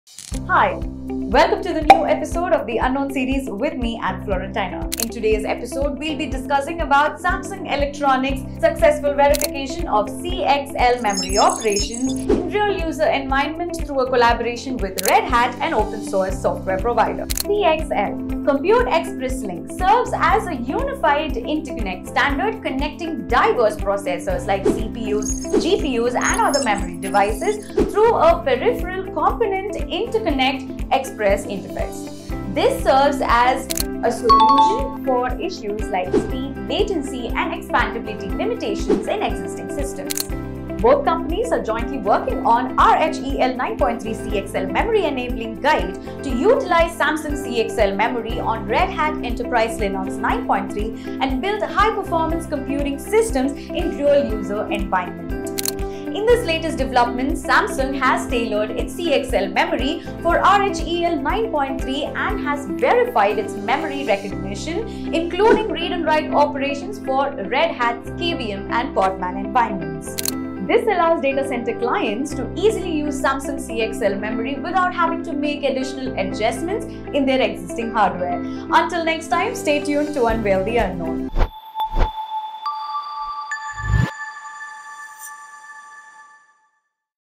The Unknown Series - Episode 8
What is CXL Memory?
Say goodbye to laggy AI & glitchy self-driving cars?
@Samsung  & @redhat (Red Hat) teamed up to use the power of CXL memory, for smoother tech experiences for everyone?

Interesting? Exciting? Intriguing?
Then Comment & Share.